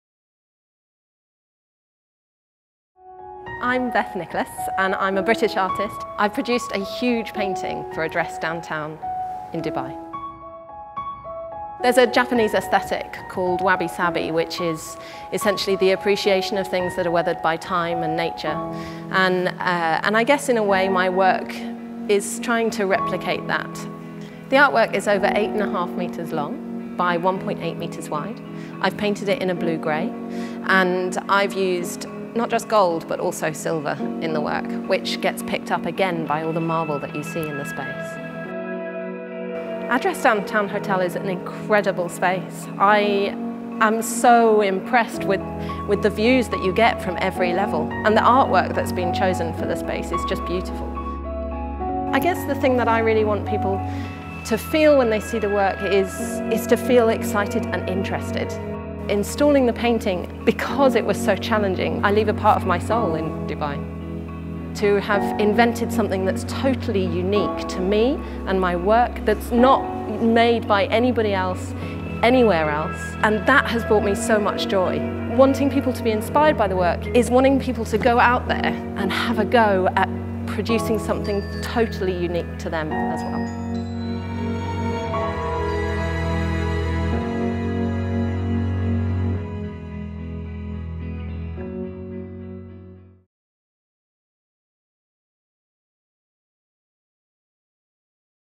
Address Downtown | Beth Nicolas | Extended Version
Meet British artist Beth Nicholas. Taking inspiration from the Japanese aesthetic of Wabi-Sabi and the appreciation of things that have been weathered by time, she has created five unique paintings that breathe life into the reception of Address Downtown. These 8.5 metres long and 1.8 metres wide painting are using blue, gold and silver colours that are reflected throughout the marble in the hotel, tying in perfectly with space. We catch up with Beth who flew in and joined us to personally work on the installation of the artwork at Address Downtown.